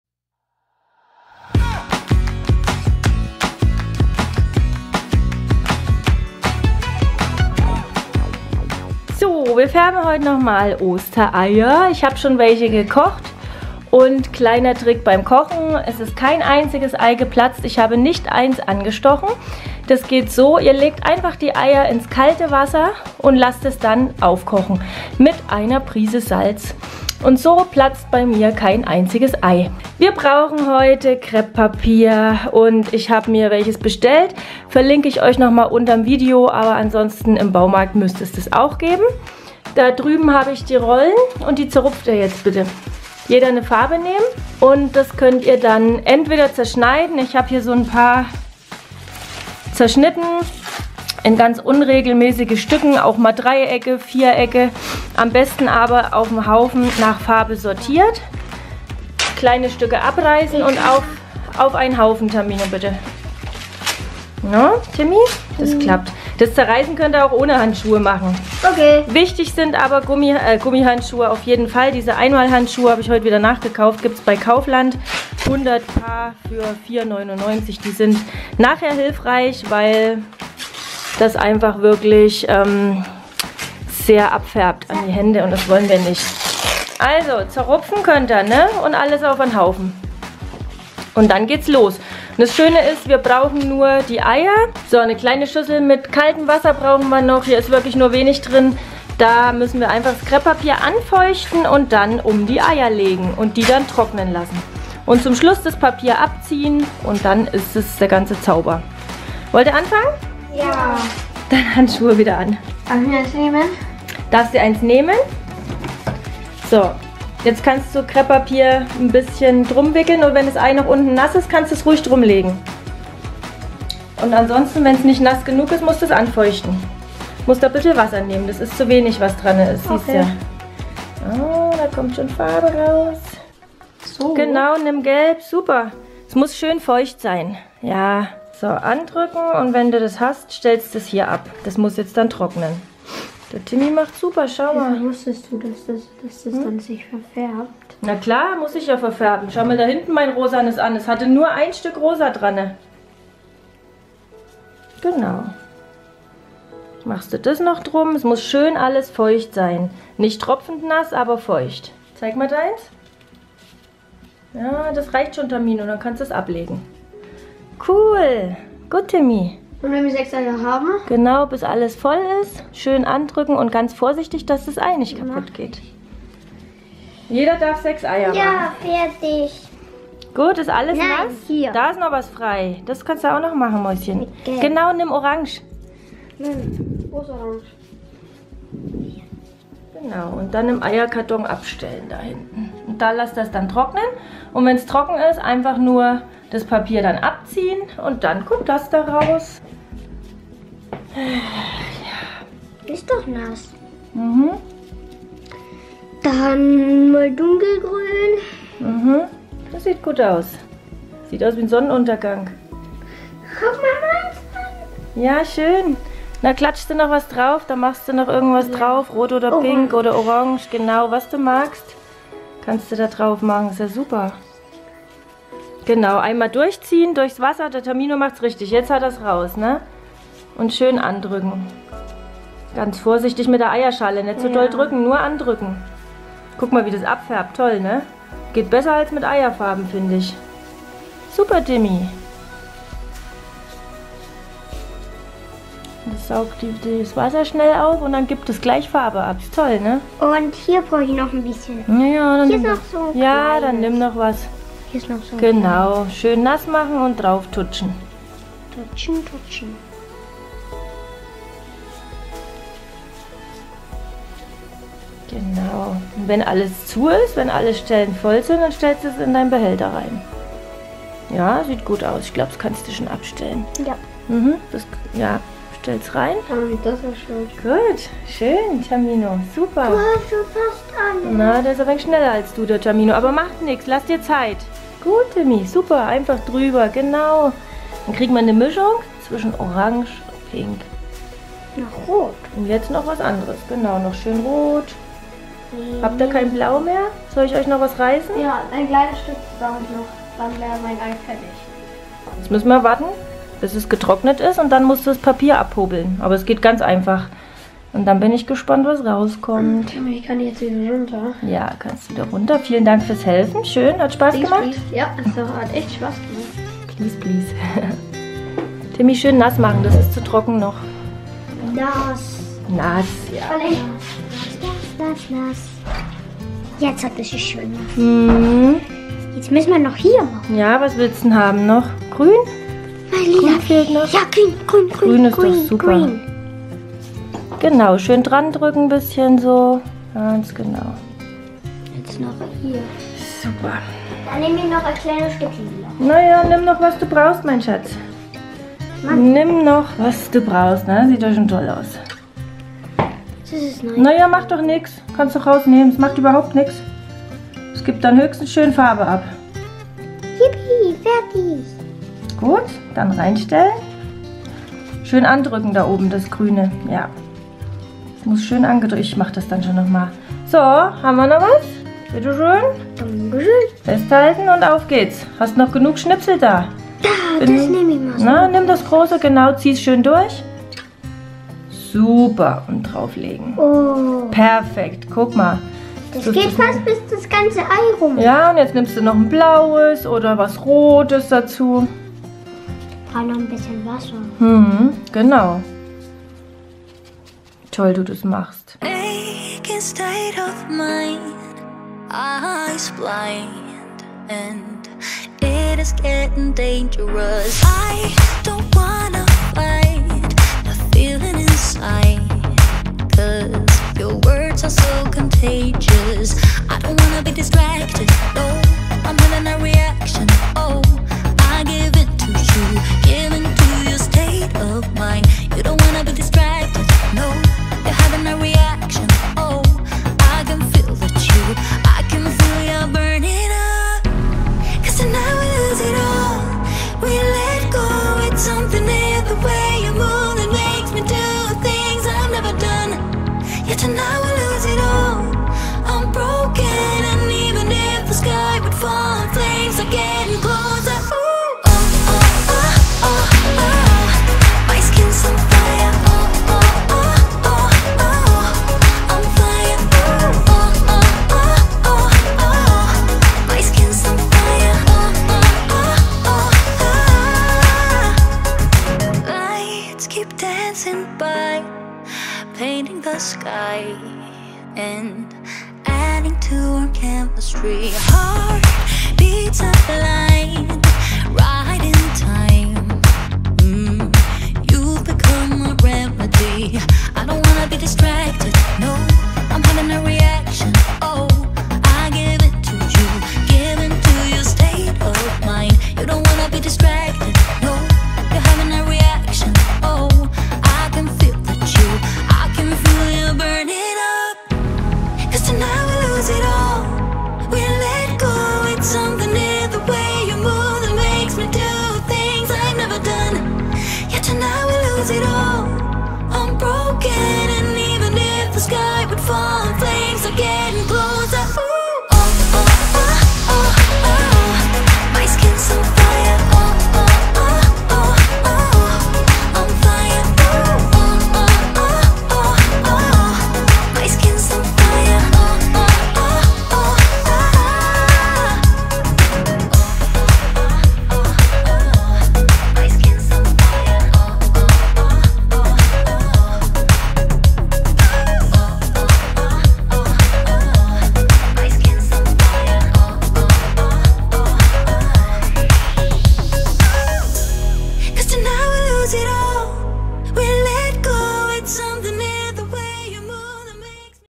OSTEREIER FÄRBEN MIT KREPPPAPIER - SCHNELL UND EINFACH - OMBRE - GALAXY
OSTEREIER FÄRBEN MIT KREPPPAPIER - SCHNELL UND EINFACH
Wie einfach das geht zeige ich Euch heute im Video. Viel Spaß

SCHALE SILBER

→ Ich schenke Dir 50 Kicks

★ NEUER KANAL
„HÄKELN und STRICKEN“

► EQUIPMENT

→ Musik - Audiobibliothek Youtube